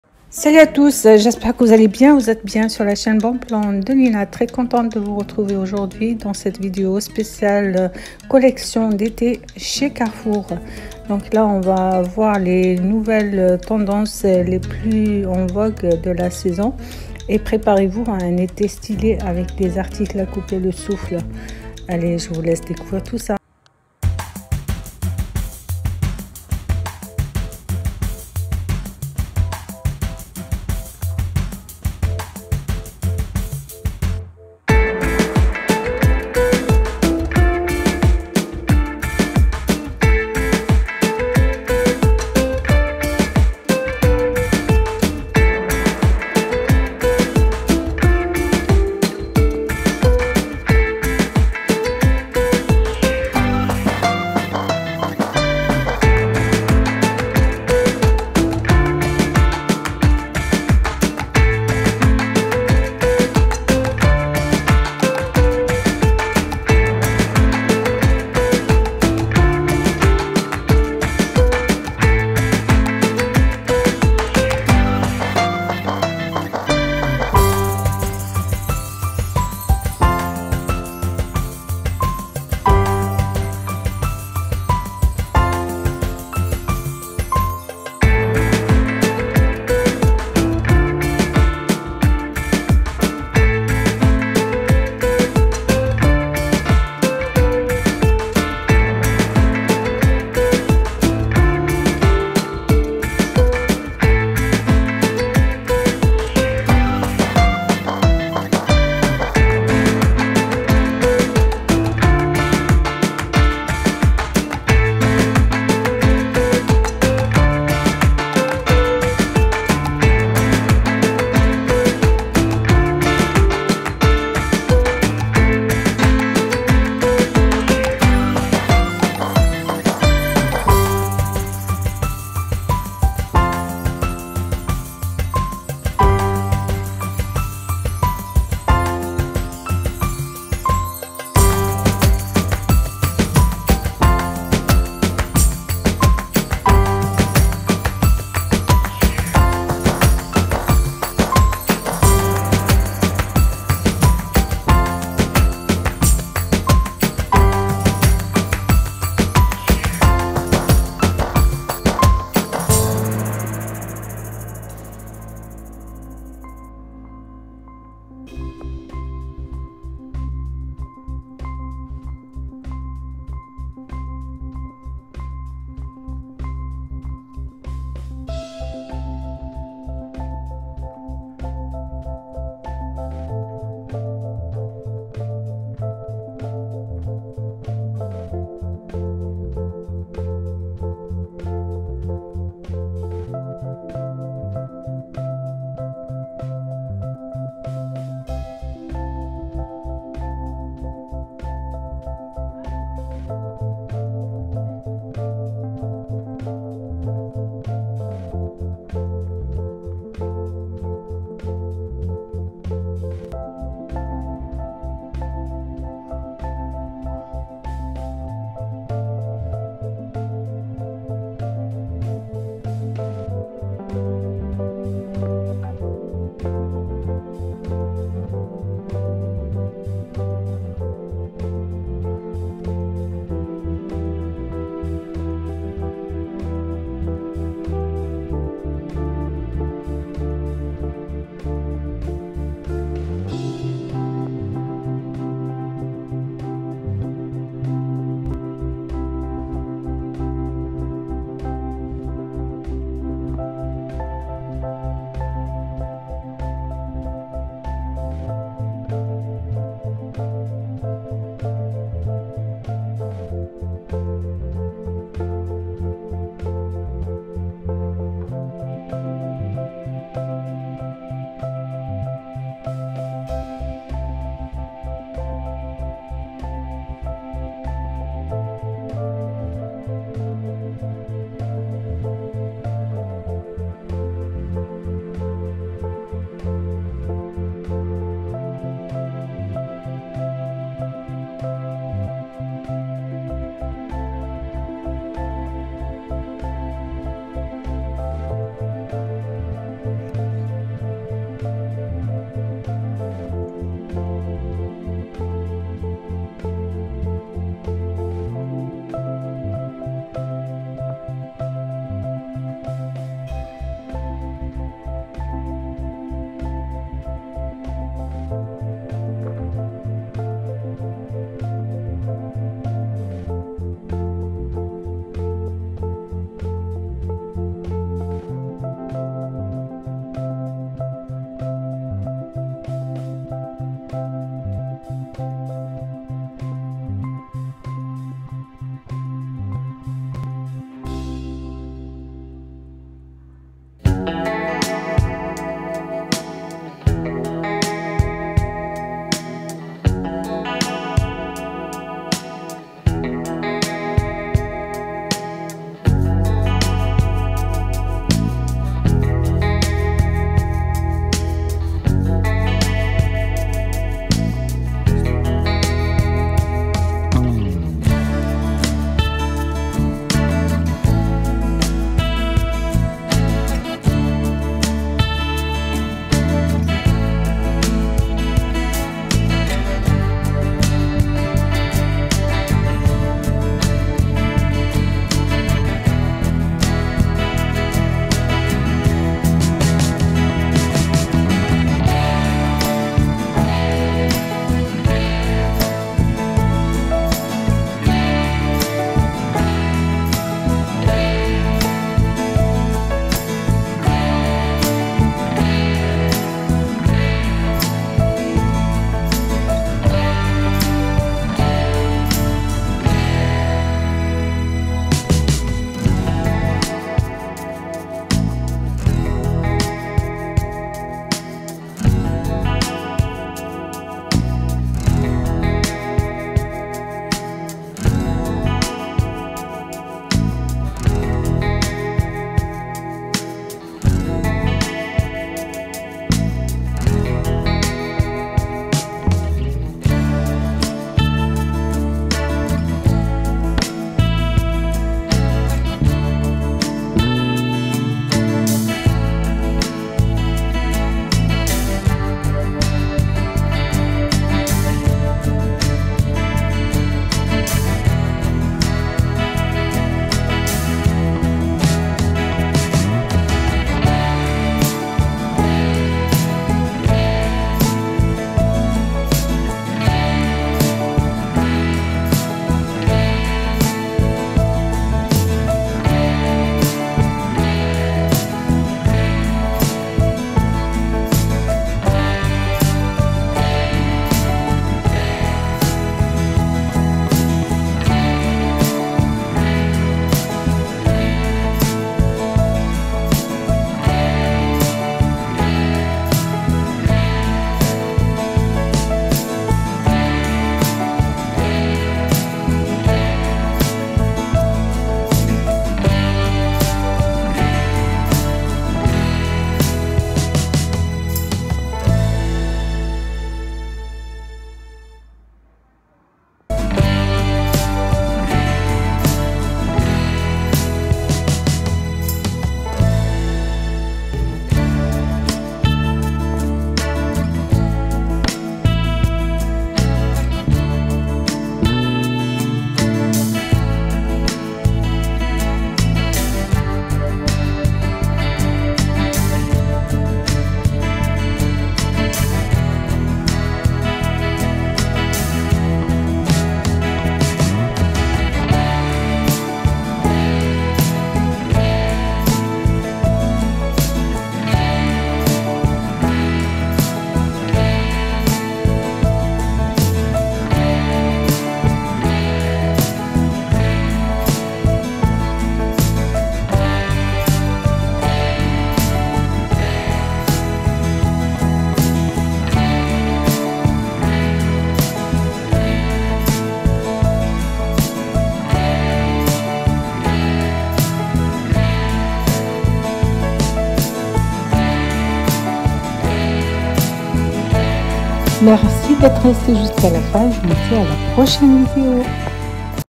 ✅ CARREFOUR NOUVELLE COLLECTION 😱 ÉTÉ 24 - OFFRES SPÉCIALES 02.05.24 VÊTEMENTS FEMMES ET HOMMES
🛍 Découvrez les offres spéciales de vêtements pour femmes chez Carrefour ! Dans cette vidéo, je vous présente les dernières tendances mode à prix abordables. Ne manquez pas les promotions et les bonnes affaires pour renouveler votre garde-robe.